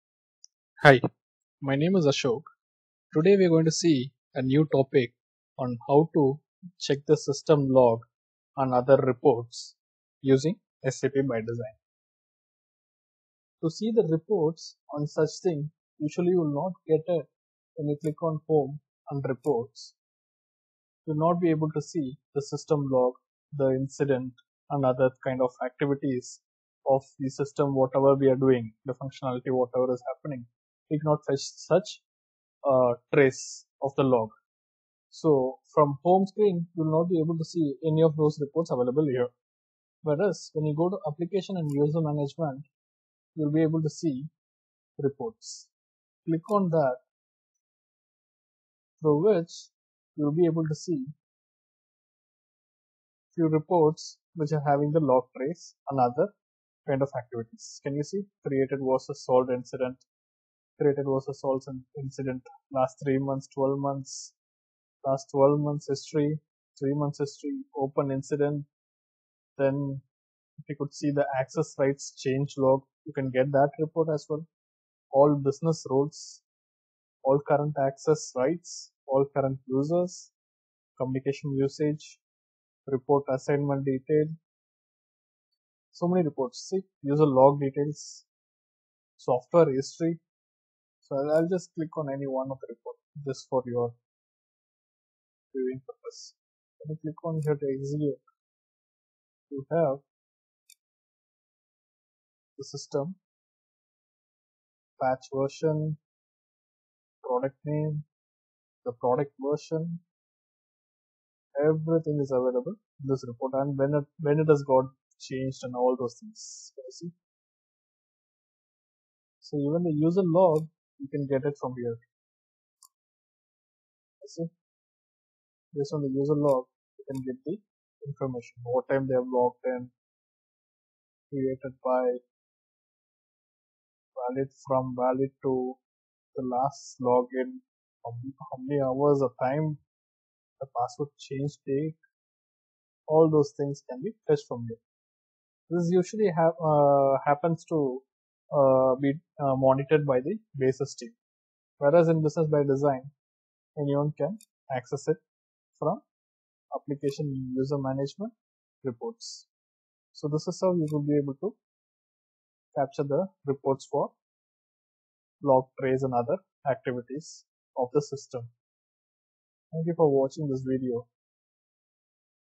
Report on Logon details in SAP Business Bydesign
In this video you will learn how to check reports on system log on, incident and version of the system and etc in report wise in SAP Business Bydesign.